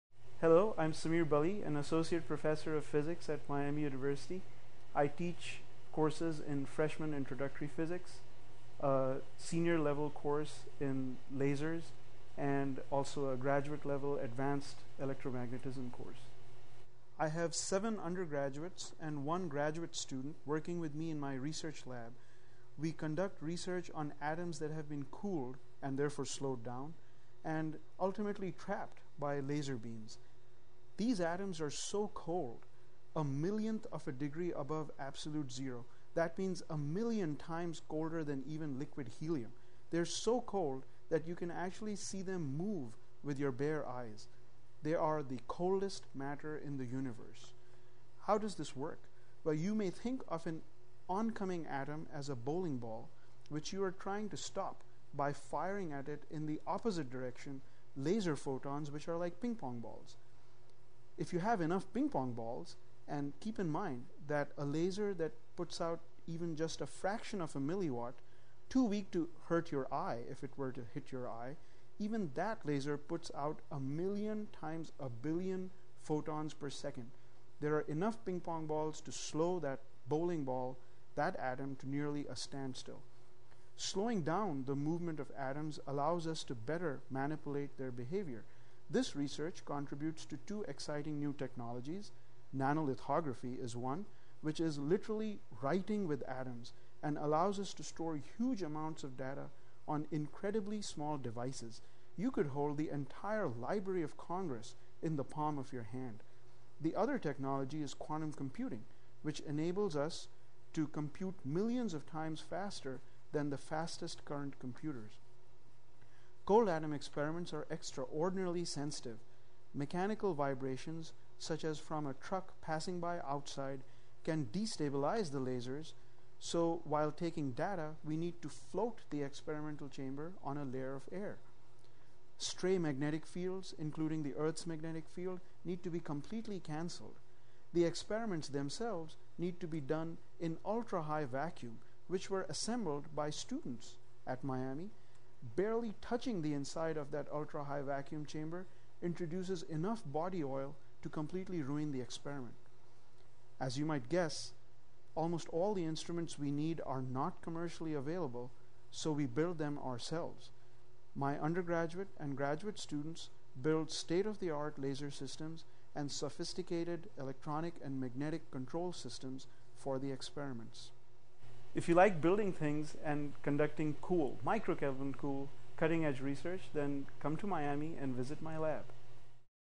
Coldest Matter in the Universe I
Miami University's Dr. Samir Bali describes the research that students conduct in his physics laboratory.
[April 2009]